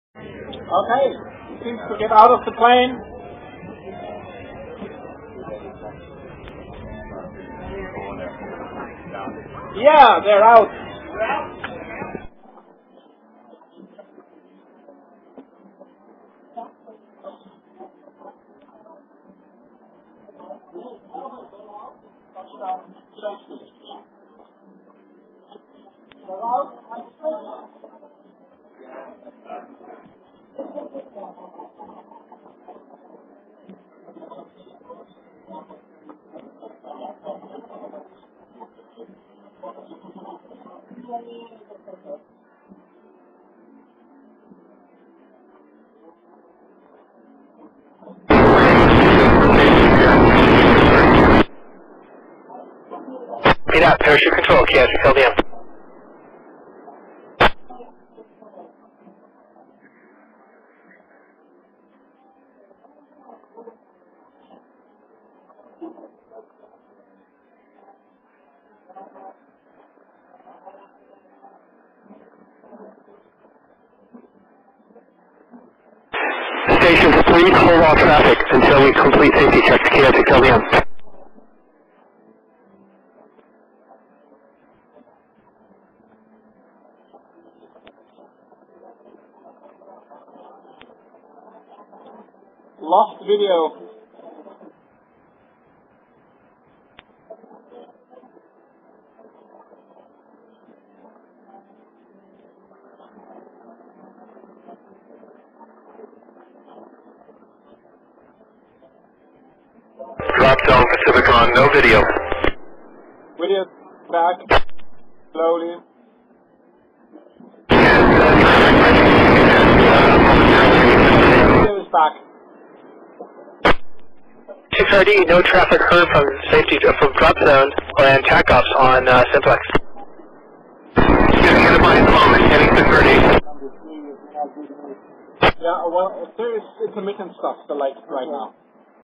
Parachute mobile mission 9 jump 2 of 3 10-16-11
Jump 2 of 3 from Parachute mobile mission 9

 Pacificon 2011 jumps

I changed the antenna configuration on this jump with rather disastrous results.

This video is only a few minutes long.